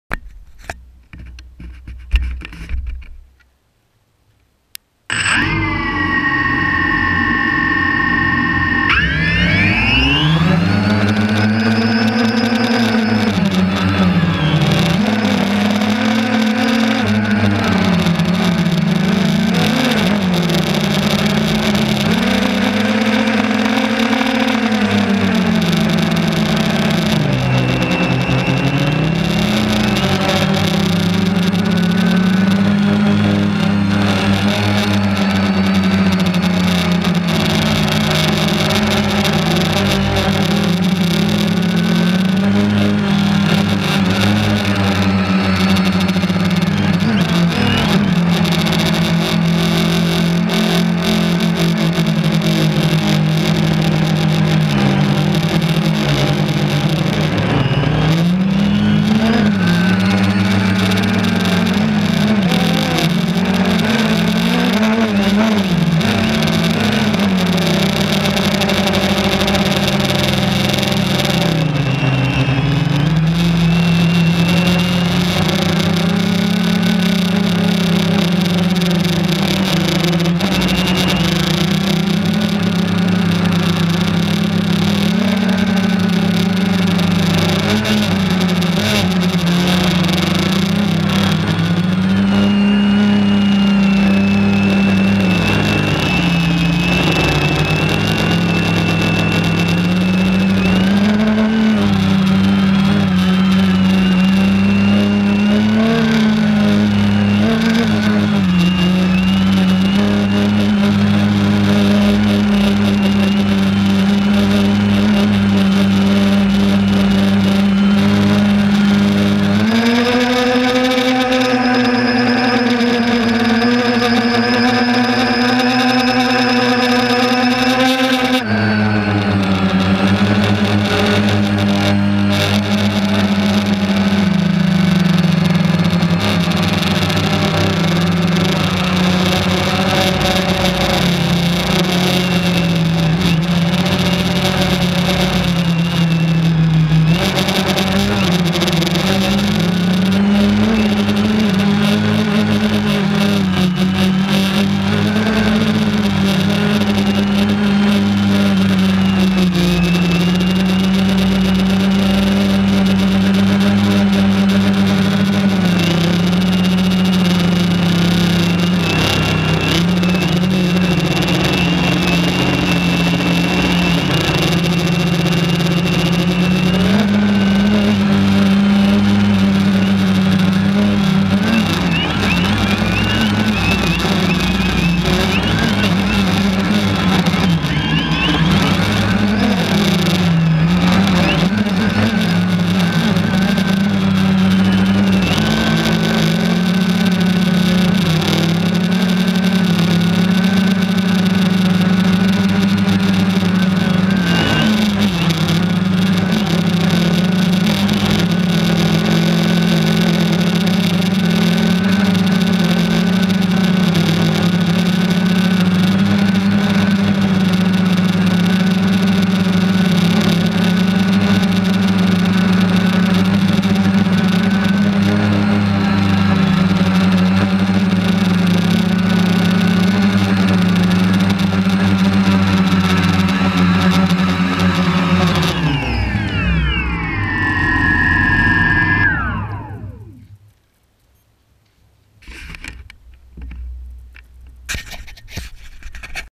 Phantom 2 1st Flight
1st Flight of my new Phantom 2 with GoPro mounted.